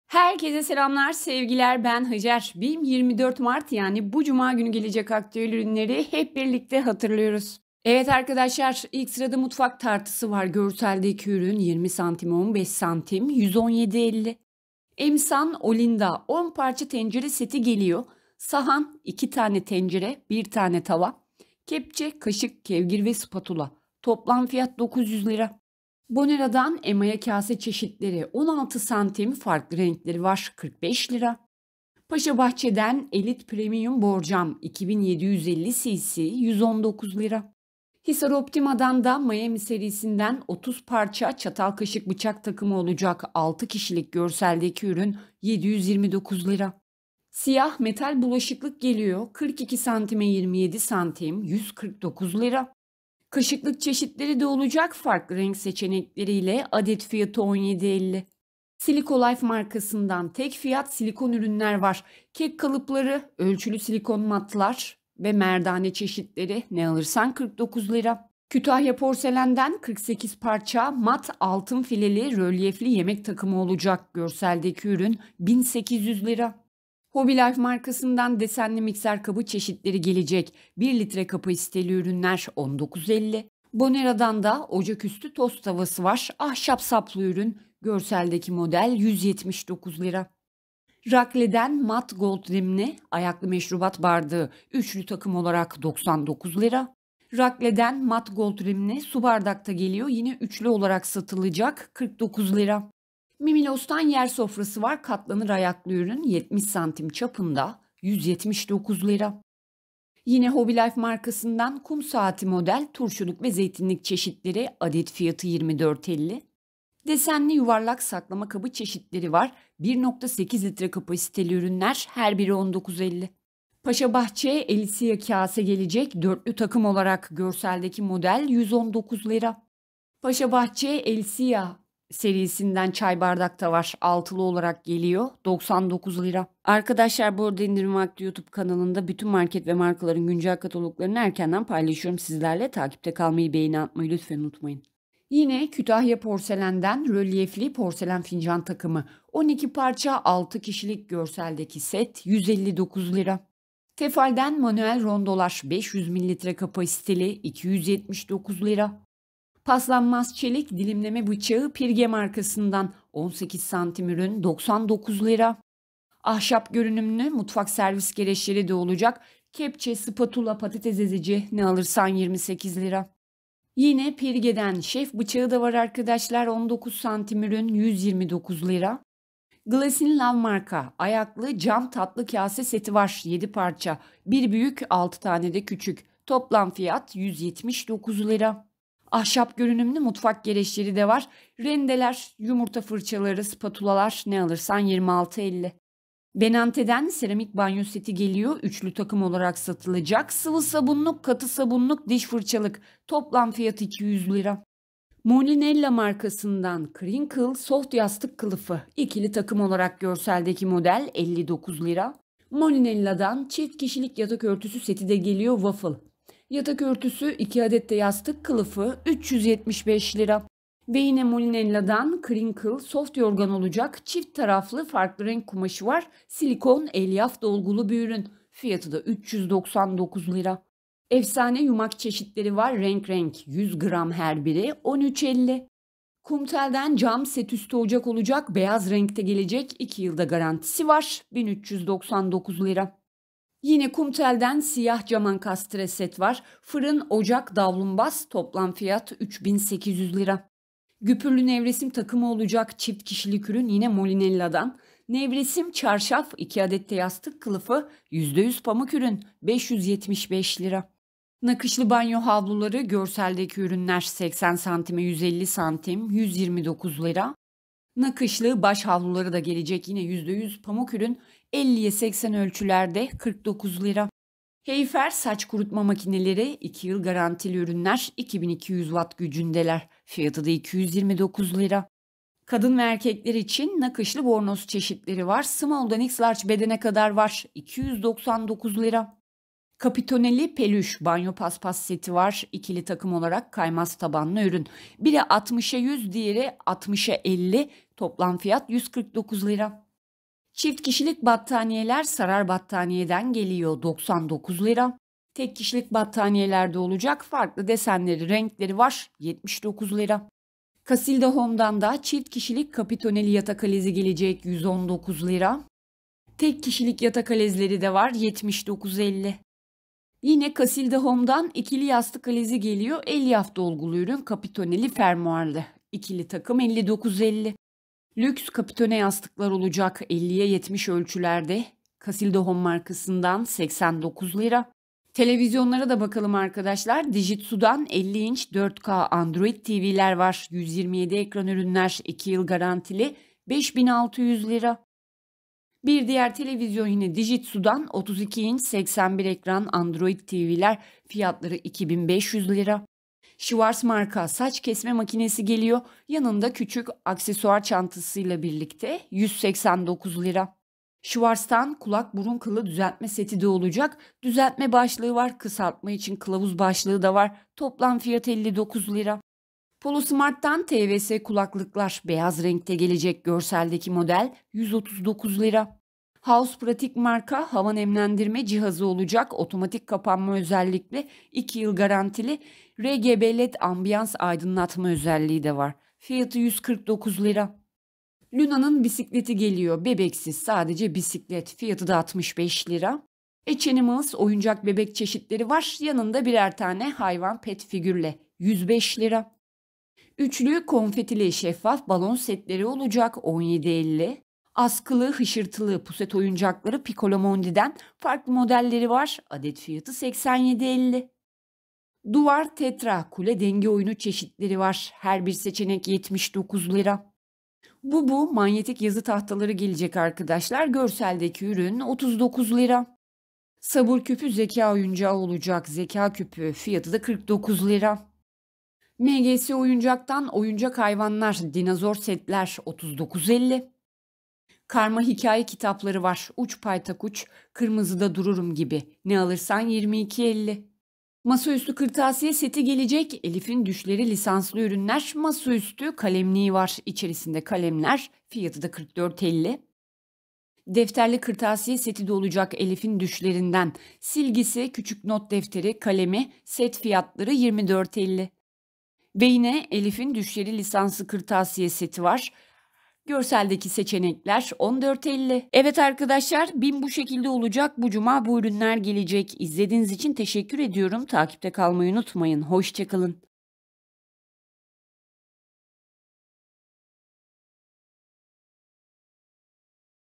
BİM ÇEYİZLİK VE EV TEKSTİLİ AKTÜEL BU CUMA | BİM MUTFAK GEREÇLERİ | 24 MART 2023 BİM KATALOĞU
Bim aktüel çeyizlik, ev ve mutfak gereçleri bu cuma geliyor! tekli sunumla tüm ürünleri 24 mart 2023 bim kataloğunu hatırlıyoruz!

Emsan Olinda 10 Parça Tencere Seti
Rakle Mat Gold Rimli Su Bardağı ~290 cc
Elysia Çay Bardağı ~170 cc,
Paşabahçe Elysia Kase ~235 cc
Kütahya Porselen
Rölyefli Porselen Fincan Takımı
Dilimleme Bıçağı ~ 18 cm
Nakışlı Bornoz Kadın/Erkek
Nakışlı Baş Havlusu 50x80 cm
Nakışlı Banyo Havlusu
2’li Yastık Alezi
Çift Kişilik Krinkıl Soft Yorgan
Krinkıl Soft Yastık Kılıfı 2’li
Lüx Kapitone Yastık
Çift Kişilik Yatak Örtüsü Seti Waffle
Tefal Manuel Rondo ~500 ml
Kütahya Porselen Mat Altın File Rölyefli 48 Parça Yemek Takımı
Hisar Optima Miami 30 Parça Çatal Kaşık Bıçak Takımı
Mutfak Tartısı ~ 20x15x1.5 cm
Sarar Battaniye Tek Kişilik Battaniye
Mimilos Yer Sofrası ~ 70cm
Mutfak Servis Gereçleri
Siyah Cam Ankastre Set
Siyah Metal Bulaşıklık ~ 9x42x27 cm
Bonera Emaye Kase ~ 16 cm
Tek Fiyat Silikon Ürünler
Elite Premium Borcam ~ 36x21 cm ~ 2750 cc
cam Ayaklı Tatlı Kase Seti
Seramik Banyo Seti
Kum Saati Turşuluk/Zeytinlik
Dijitsu 32 İNÇ ANDROID TV
50 İNÇ 4K ANDROID TV
Polosmart Tws Kulaklık
Schwartz
Saç Kesme Makinesi
House Pratic Hava Nemlendirme Cihazı